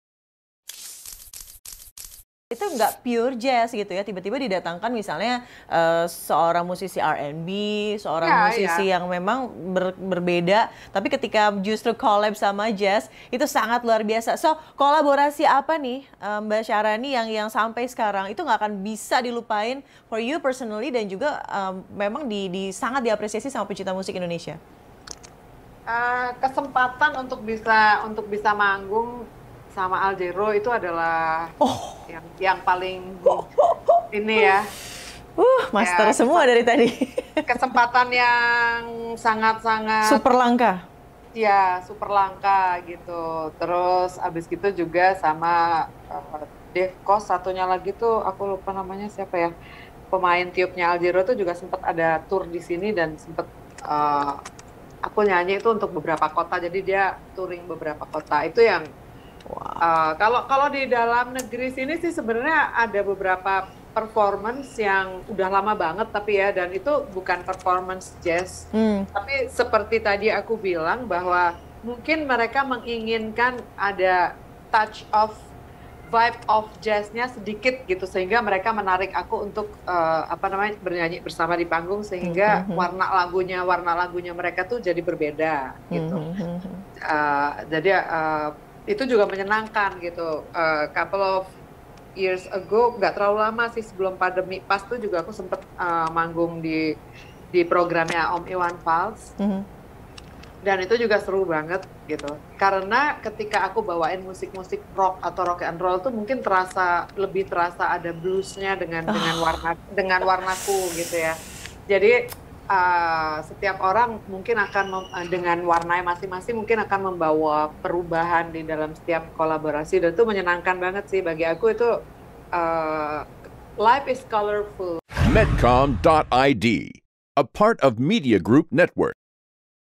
Kolaborasi Manggung Bersama Al JarreauTak Bisa Syaharani Lupakan
Syaharani atau yang memiliki nama lengkap Saira Syaharani Ibrahim adalah seorang penyanyi jazz. Karirnya di dunia tarik suara diawali pada tahun 1990. Tentu pengalaman manggungnya sudah tidak diragukan lagi. Dari sekian pengalamannya, berkolaborasi dengan Al Jarreau merupakan hal yang tak terlupakan baginya.